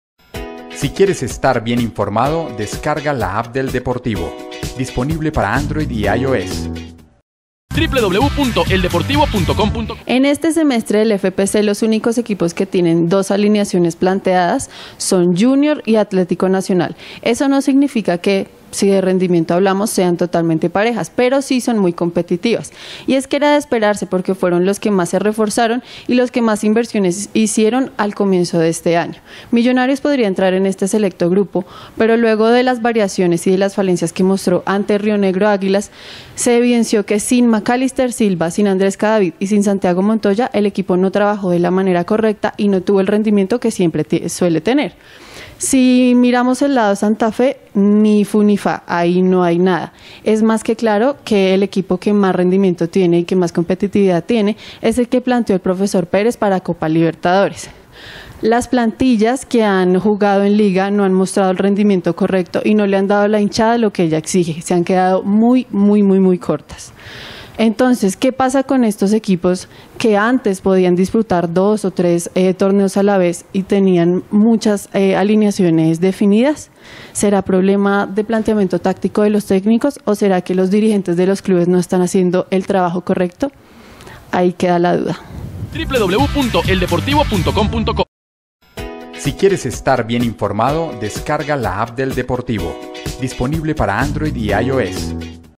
Era de esperarse lo de Nacional y Junior
Una importante diferencia ha quedado al descubierto después de los primeros partidos de la Liga Colombiana y la Copa Libertadores.